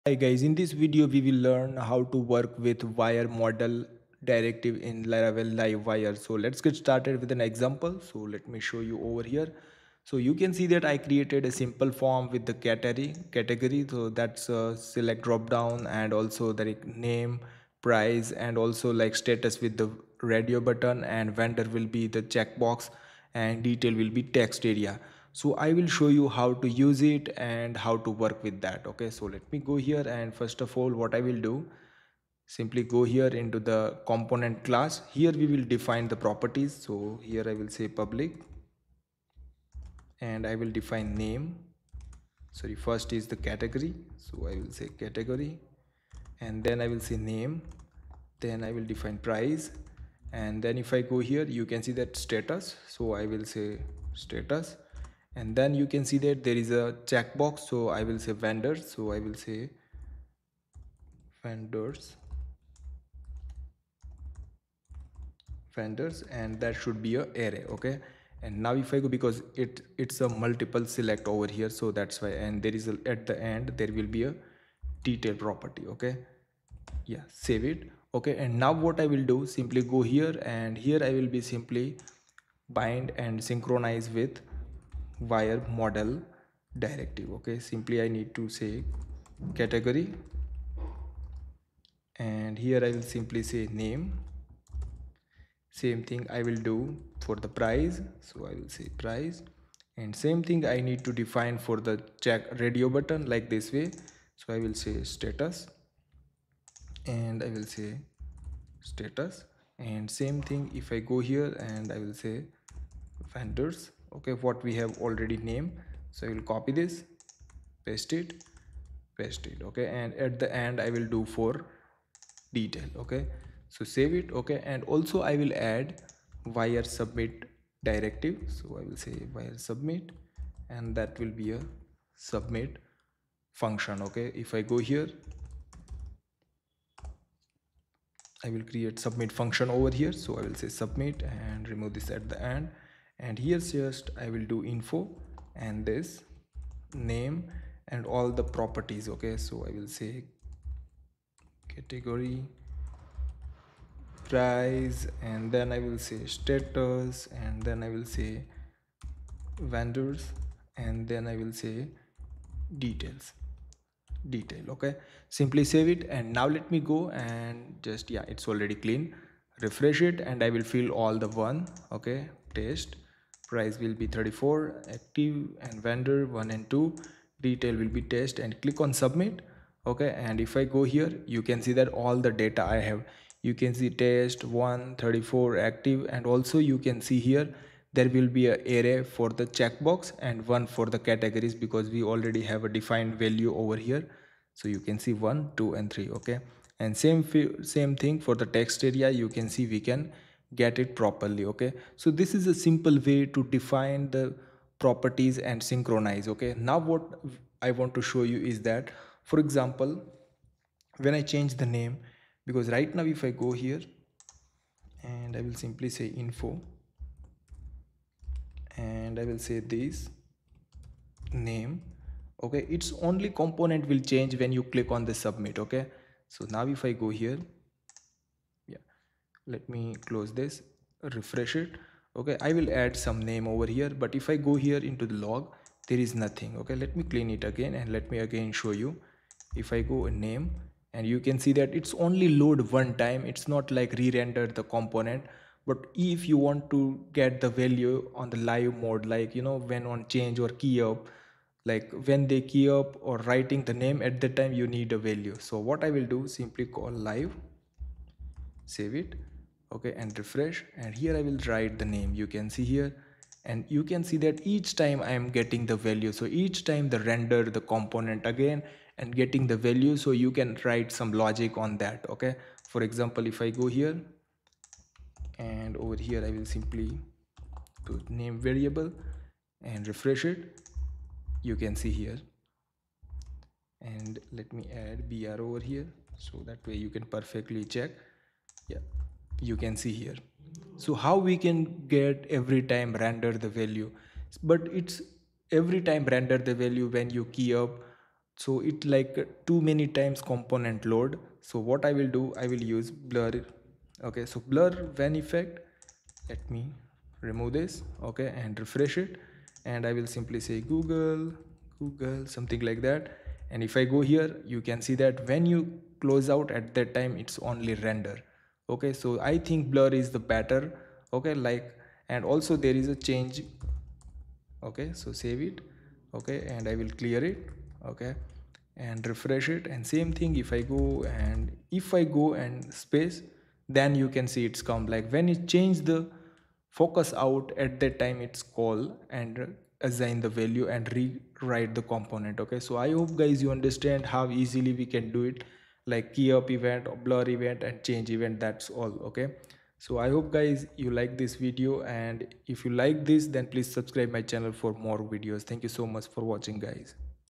wire:model | Bind Form Input in Laravel Livewire 3 | EP 12
In this video, i will show you how to bind inputs using wire:mode directive in laravel liveiwre 3.